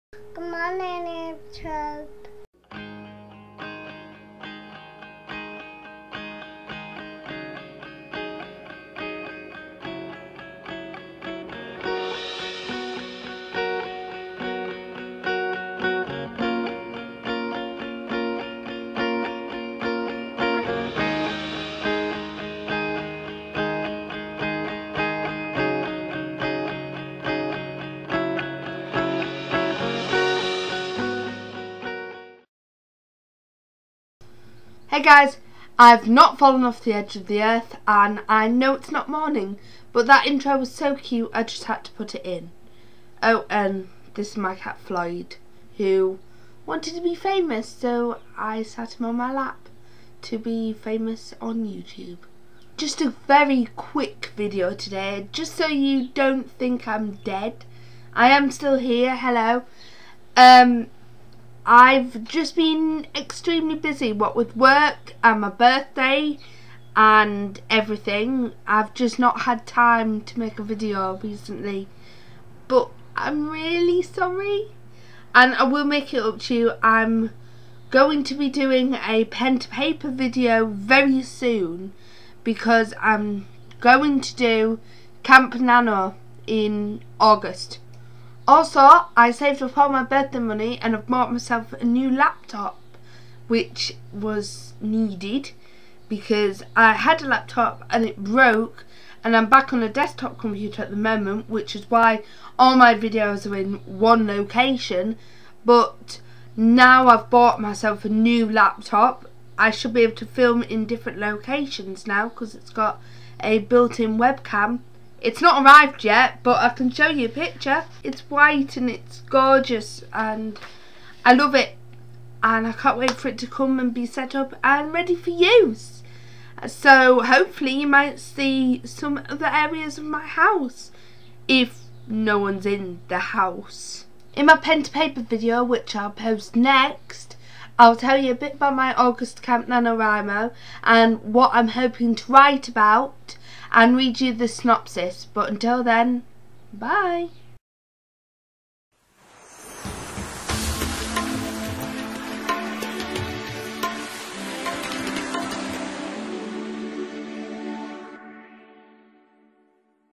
The I'm Not Dead Video!
I havent made a video in a while so i needed to make an update video or more commonly known as the 'I'm Not Dead Video'